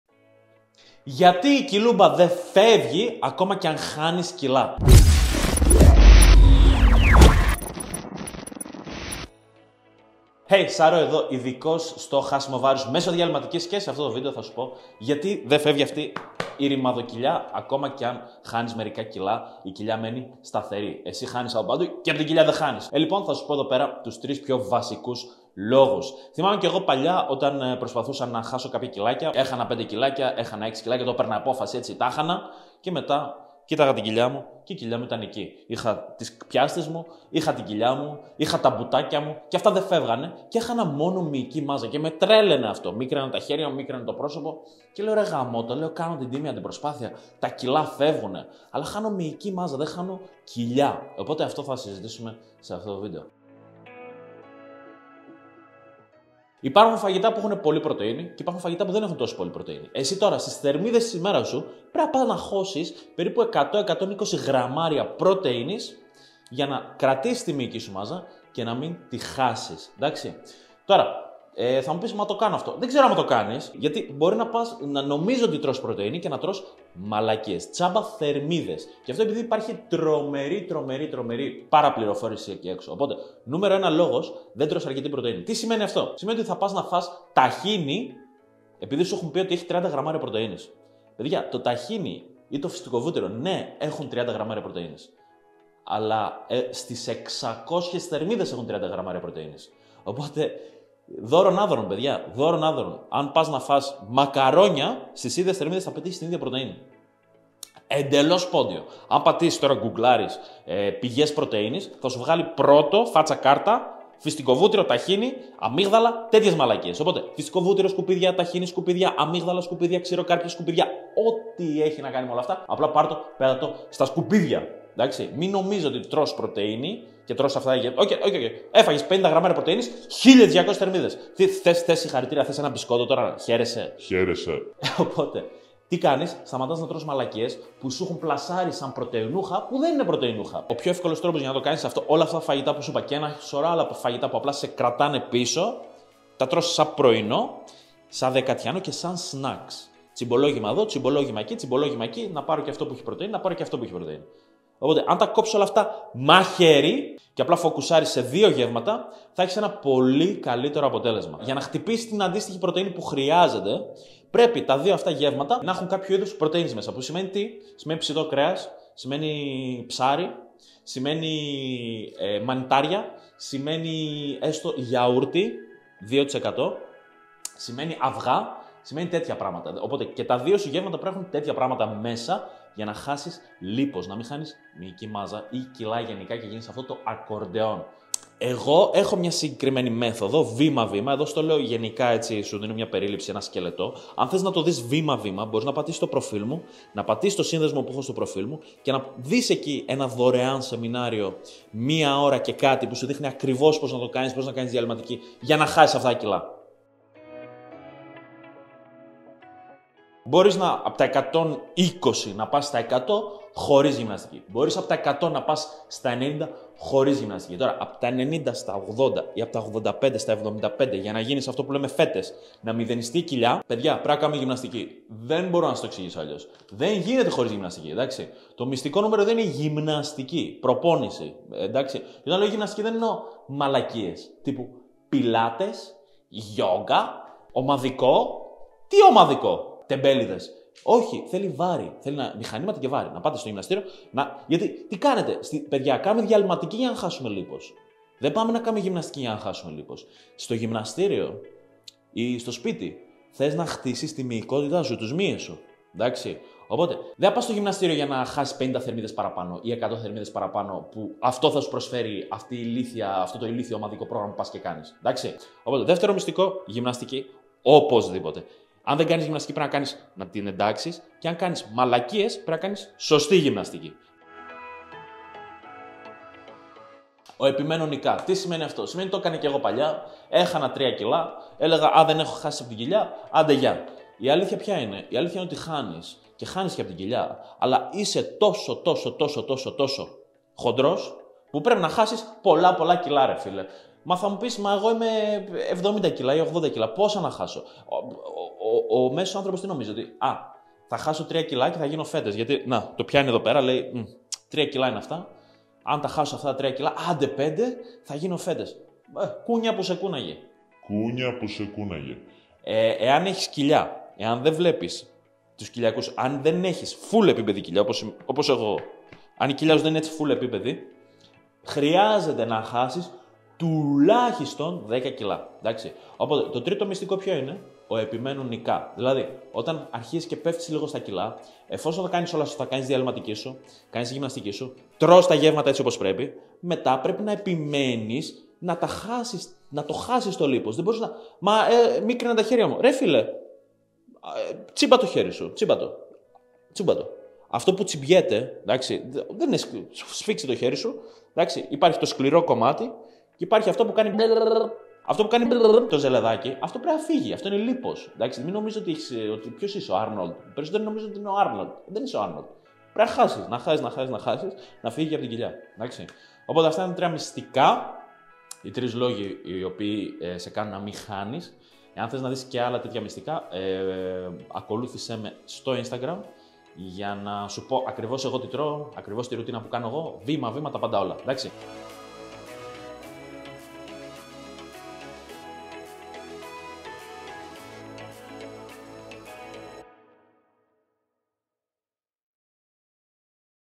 Γιατί Δε Φεύγει Η Κοιλιά Ενώ Χάνεις Κιλά; - Sharoseto Fitness
Τα ήξερες όλα αυτά;

Πως Έχασα 10 Κιλά Λίπους Σε 3 Μήνες Μέσω Της Διαλειμματικής Νηστείας Νηστείας

Στείλε μου απορίες στο ινστα